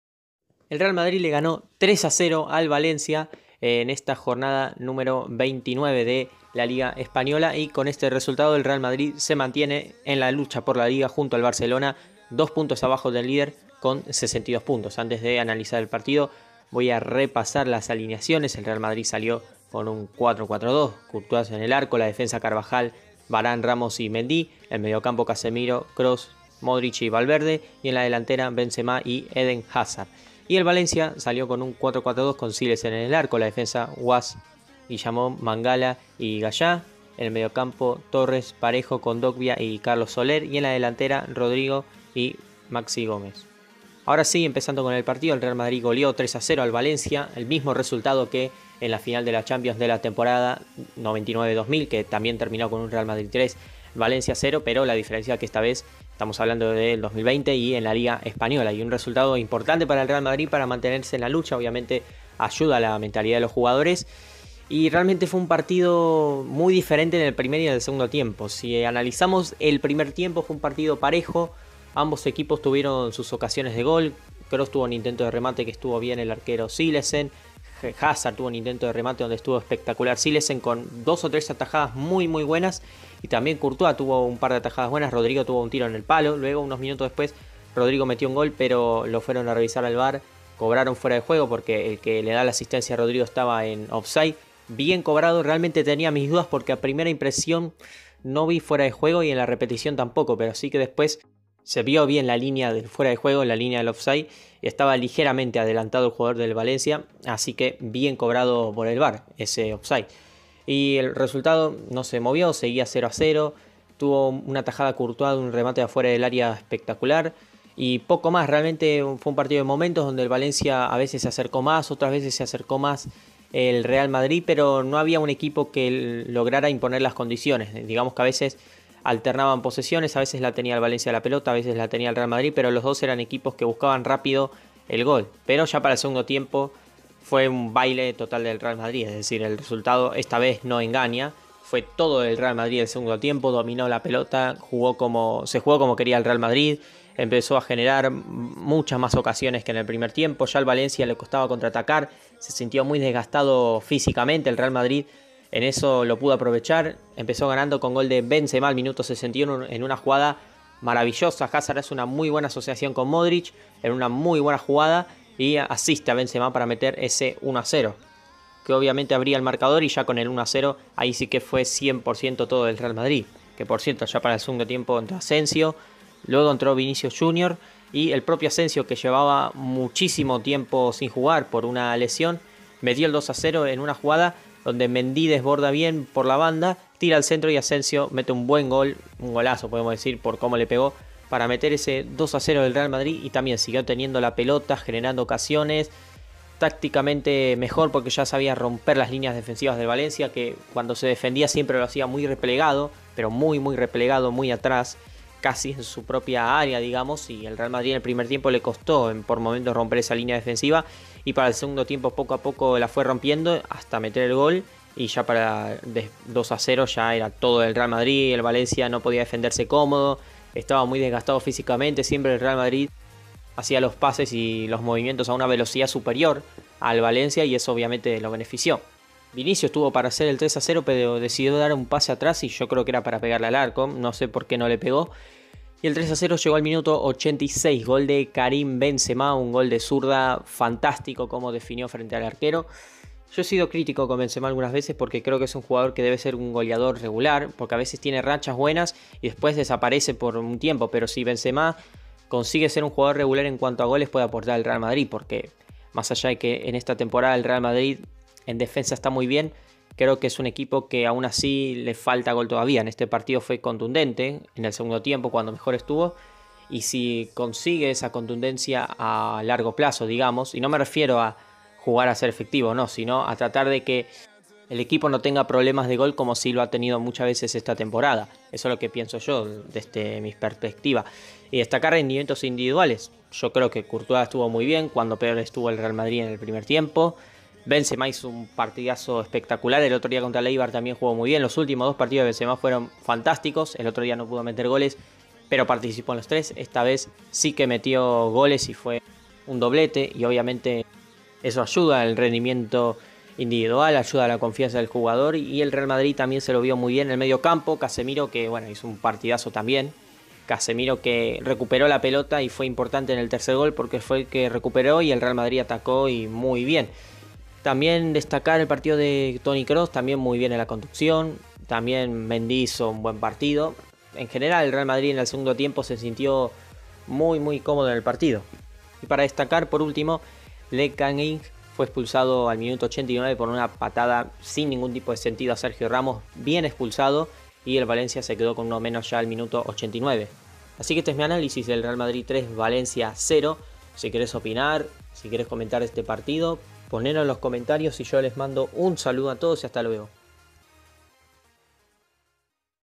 REAL MADRID 3-0 VALENCIA / MI OPINIÓN Y ANÁLISIS
El Real Madrid goleó 3 a 0 al Valencia, al igual que en la final de la Champions 1999/20, con la diferencia de que esta vez fue en la liga. Marcaron Benzema (x2) y Asensio para el conjunto de Zidane que se mantiene en la lucha por la liga.